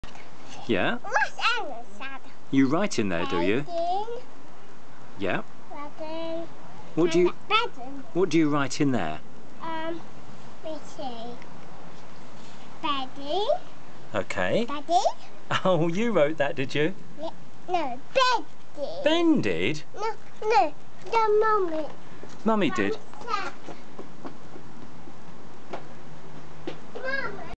Charlotte 12 Aug 2011 002.MOV
On Friday evening 12 Aug, Charlotte had just returned from day nursery and couldn't wait to show Pete her new bag and what she kept in it. Very proud of it she is.